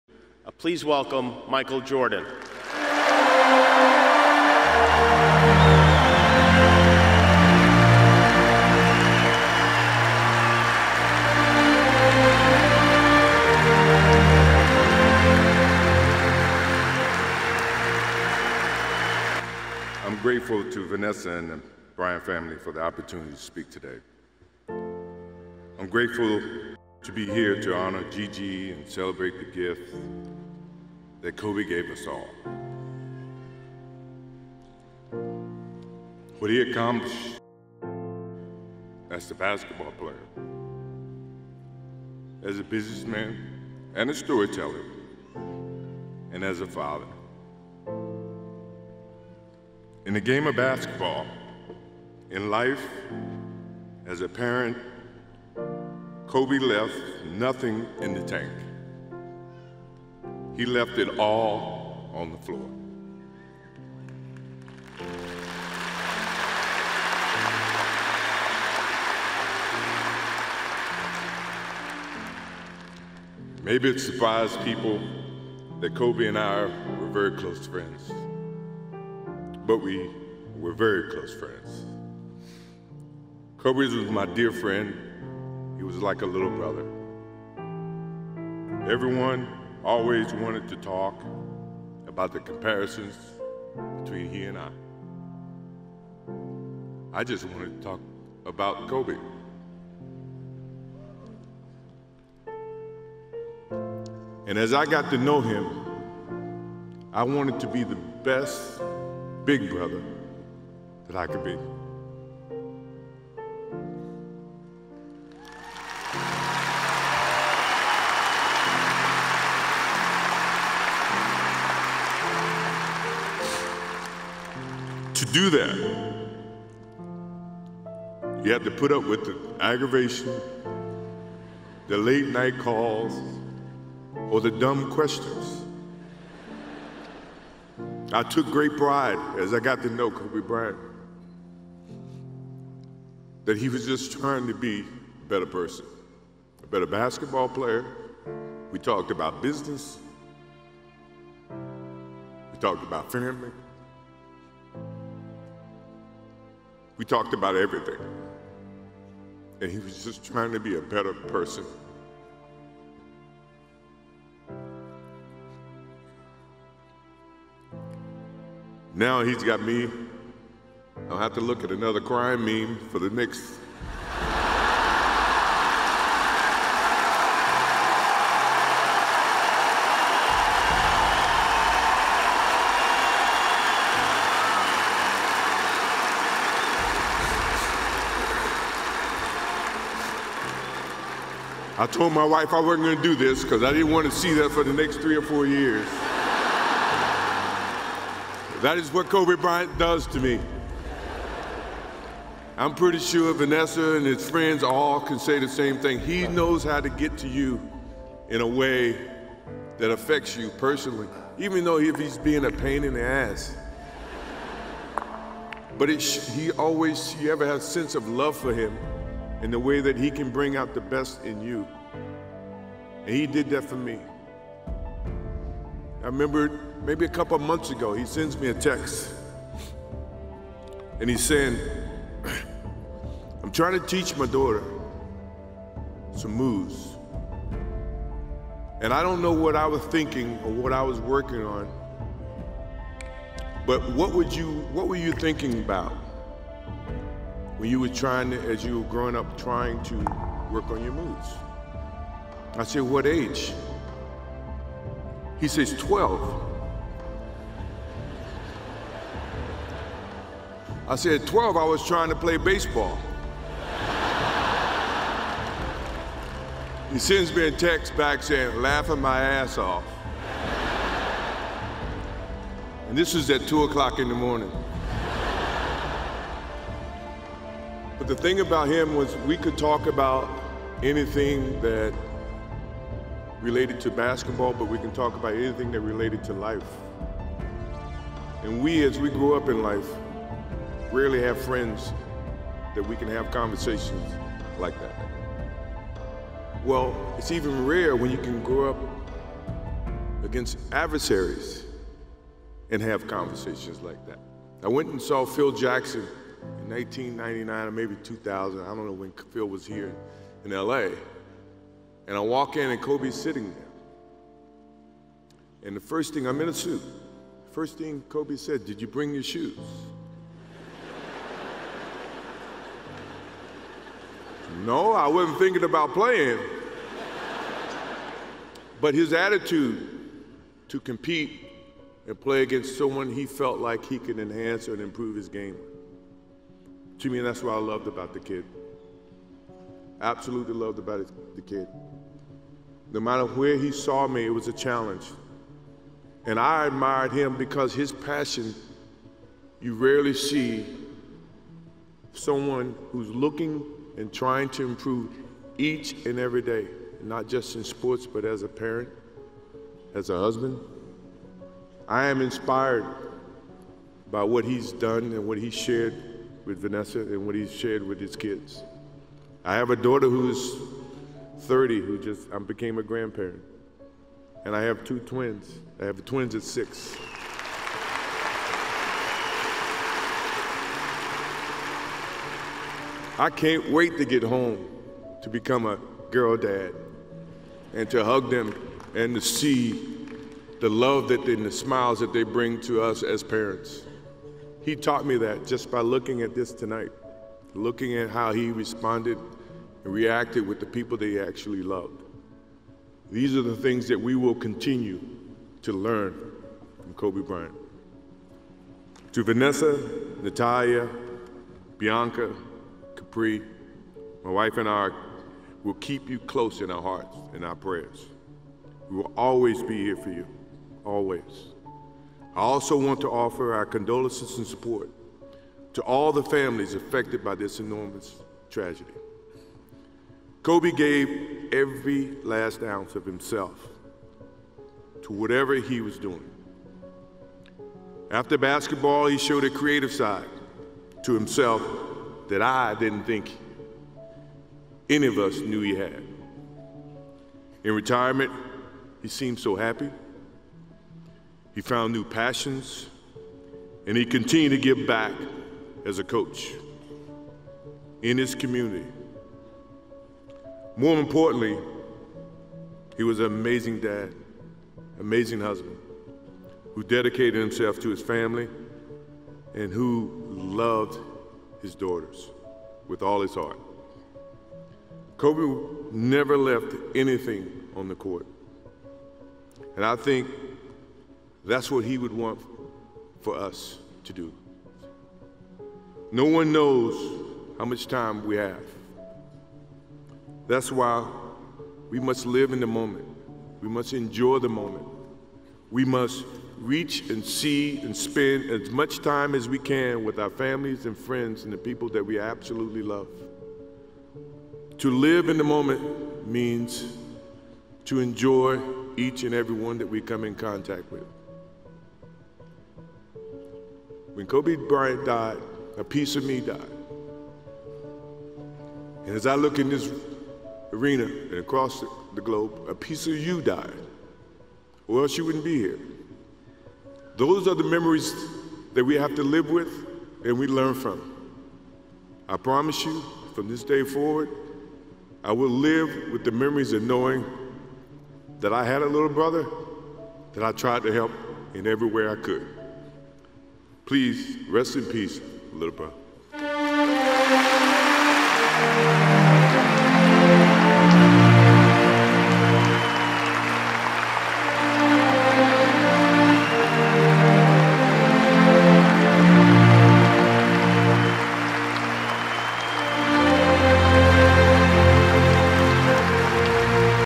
Michael Jordan recuerda con lágrimas a Kobe Bryant
"Cuando Kobe Bryant murió, murió un pedazo de mí" - Michael Jordan llora al recordar a su "hermano pequeño".

NB is a digital media website that focuses on Latin American politics.